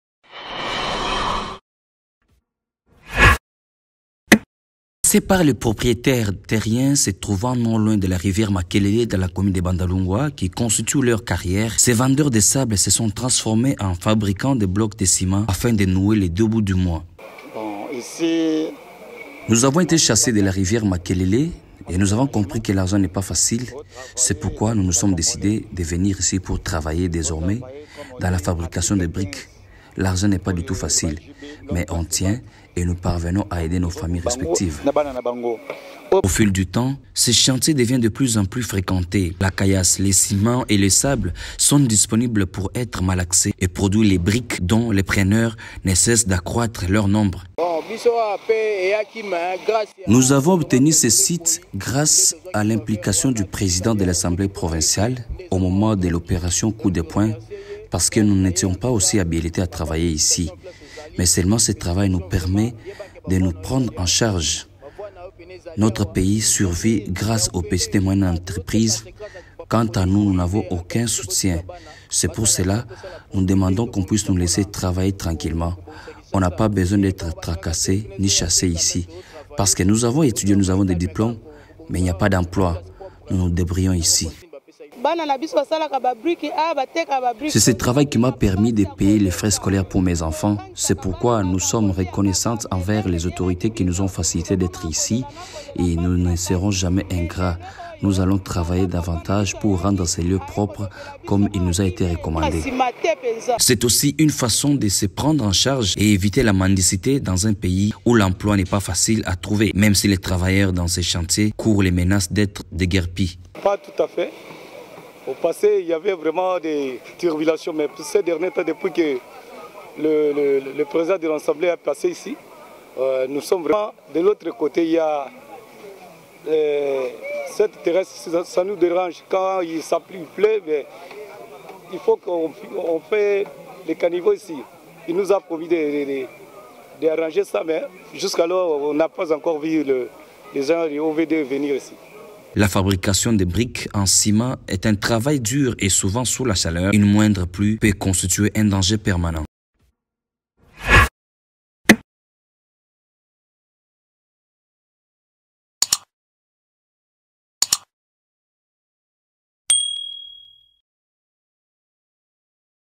RD-Congo: ils vivent de la fabrication manuelle de parpaings en ciment
Chassés par les propriétaires terriens se trouvant non loin de la rivière Makelele, qui constituait leur carrière, ces vendeurs de sable se sont transformés en fabriquant des blocs de ciment afin de nouer les deux bouts du mois.